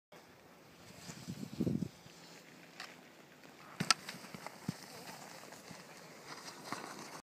360 by Marat
Night skiing @ central part in Summit at Snoqualmie